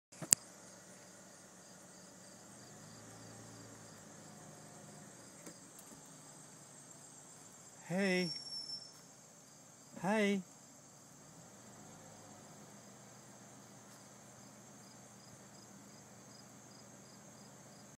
Deer at Stone Mountain State Park
Deer by the road at Stone Mountain State Park.  They acted quite tame.